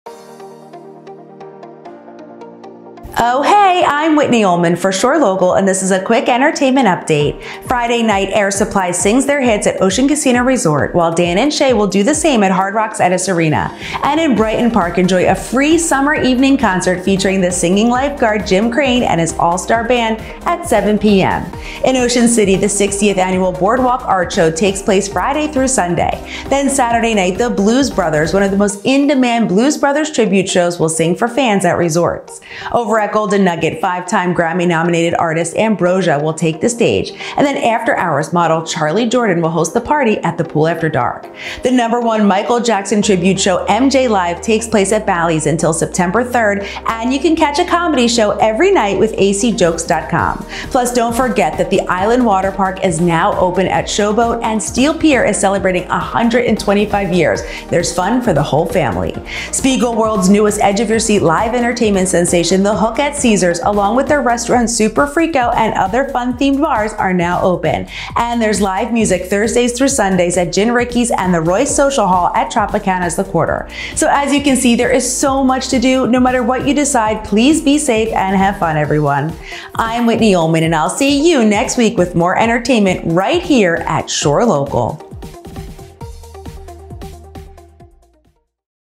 Jersey Shore Entertainment Whitney’s Shore Local Picks | Air Supply, Dan + Shay, The Blooze Brothers
ohheyyy I’m Whitney Ullman for Shore Local Newsmagazine and this is a quick entertainment update.
Friday night Air Supply sings their hits at Ocean Casino Resort, while Dan + Shay will do the same at Hard Rock Hotel & Casino Atlantic City’s Etess Arena.

𝑩𝒓𝒊𝒈𝒉𝒕𝒐𝒏 𝑷𝒂𝒓𝒌 𝑬𝒗𝒆𝒏𝒕 𝑷𝒐𝒔𝒕𝒑𝒐𝒏𝒆𝒅.

In Ocean City, the 60th annual boardwalk art show takes place Friday - Sunday. Then Saturday night, The Blooze Brothers, one of the most in-demand blues brothers tribute shows will sing for fans at Resorts Casino Hotel. Over at Golden Nugget Atlantic City, 5-time Grammy Nominated artists Ambrosia, will take the stage, and then after hours Model Charly Jordan will host the party at The Pool After Dark at Harrah's Resort Atlantic City.

The number one Michael Jackson Tribute show, MJ Live takes place at Bally's Atlantic City until September 3rd. And you can catch a comedy show every night with acjokes.com ACJokes.com Comedy Clubs.

Plus, don’t forget the new Island Waterpark at Showboat Hotel Atlantic City, and Steel Pier is celebrating 125 years, so make sure to check their specials, from their food to attractions. It’s fun for the whole family!

Spiegelworld’s newest edge of your seat live entertainment sensation, The Hook, at Caesars Atlantic City Hotel & Casino, along with their restaurant Superfrico, and other fun themed bars is now open. And there is live music Thursday’s - Sunday’s at Gin Rickey's  and The Royce AC at Tropicana’s The Quarter.

As you can see, there’s so much to do, no matter what you decide, be safe and have fun everyone.